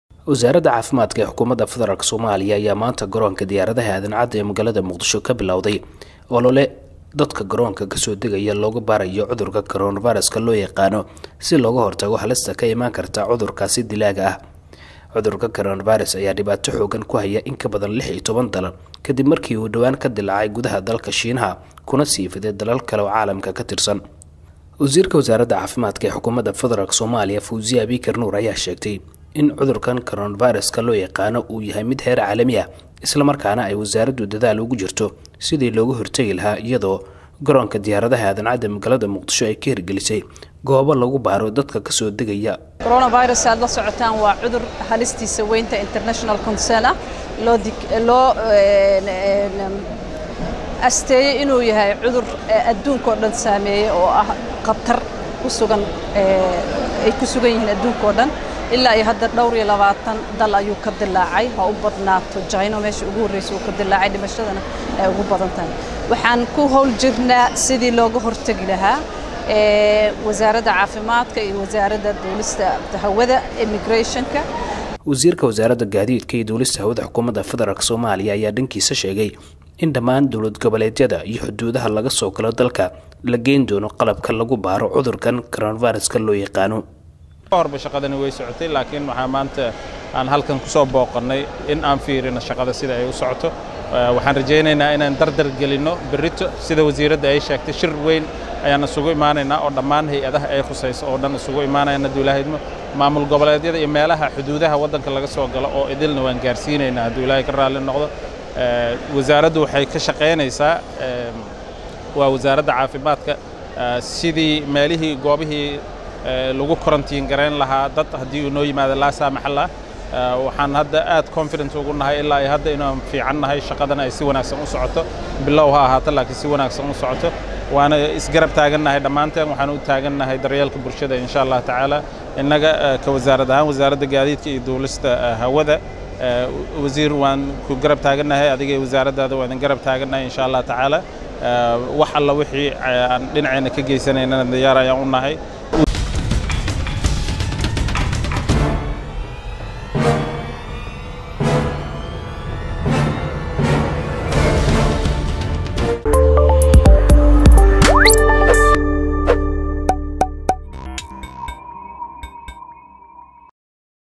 Sequence 19
Wasarada caafimadka soomaliya oo hirgilisay xarunta lagu baaro cudurka caronaviros